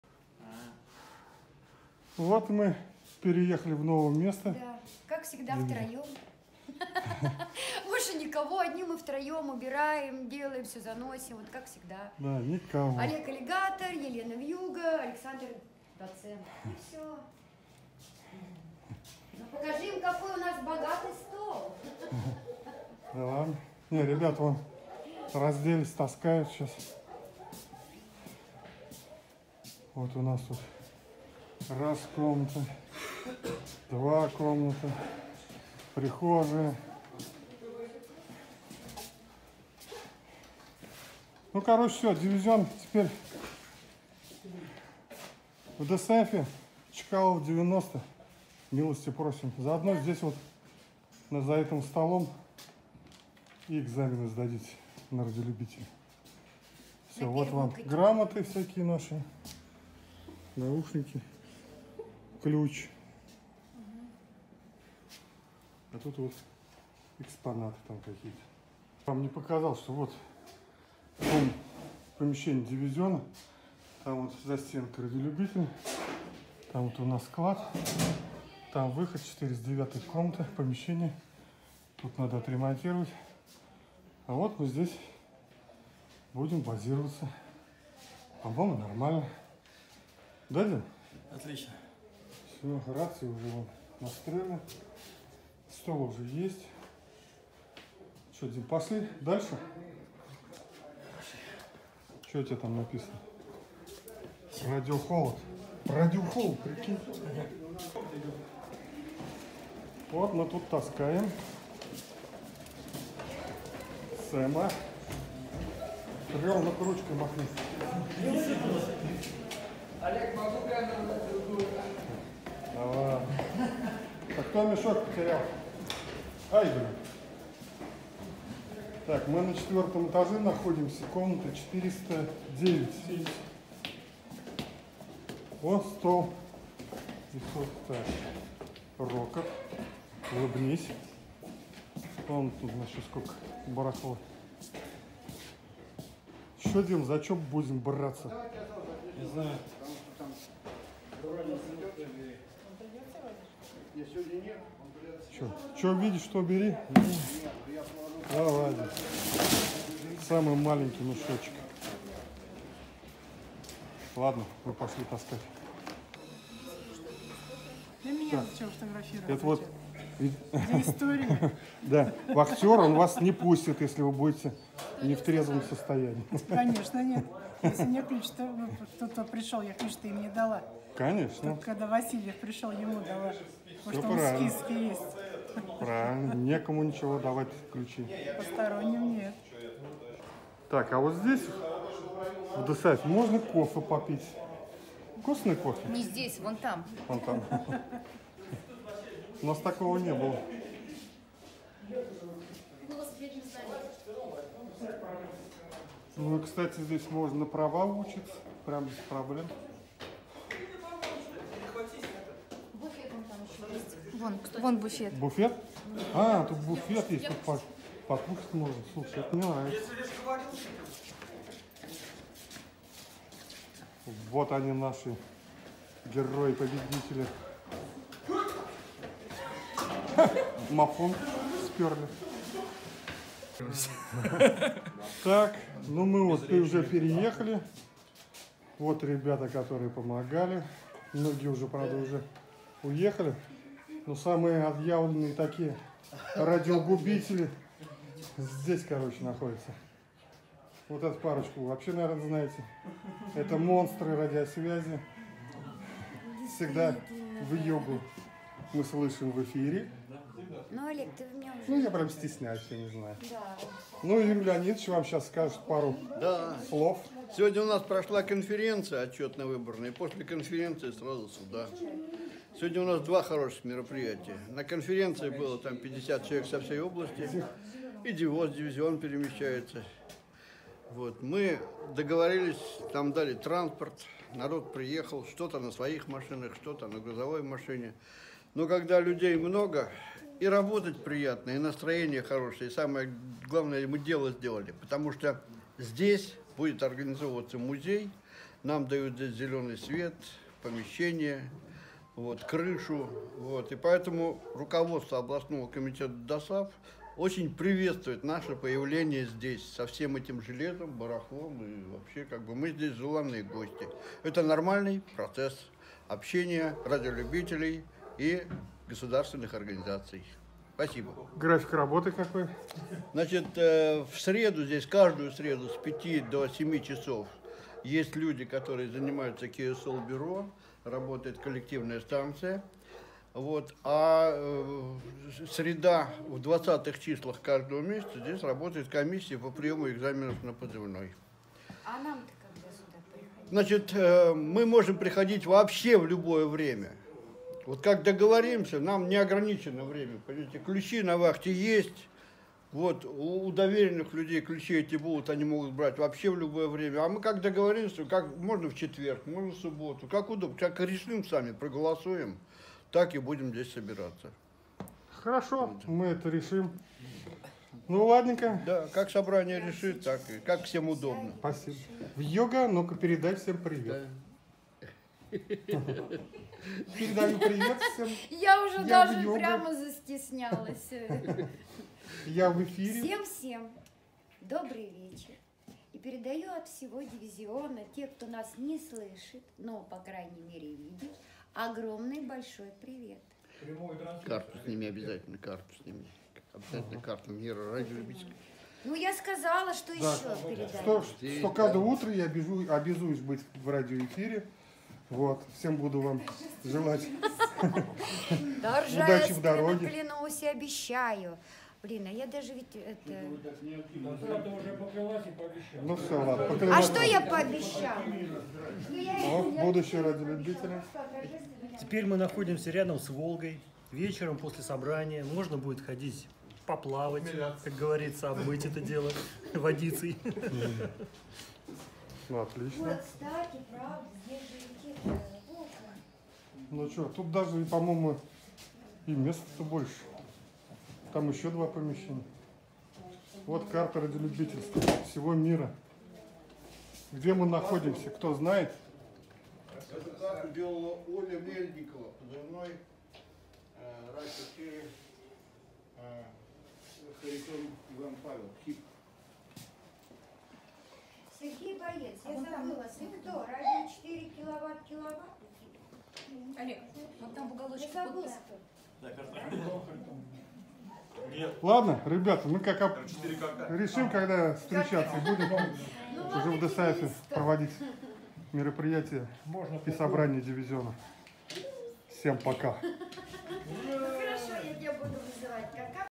Дивизион переехал в Досааф. 14.12.2019
Клуб Дивизион переехал в Досааф по адресу: Самара, Чкалова,90. График встреч Дивизиона в Досааф будет позже...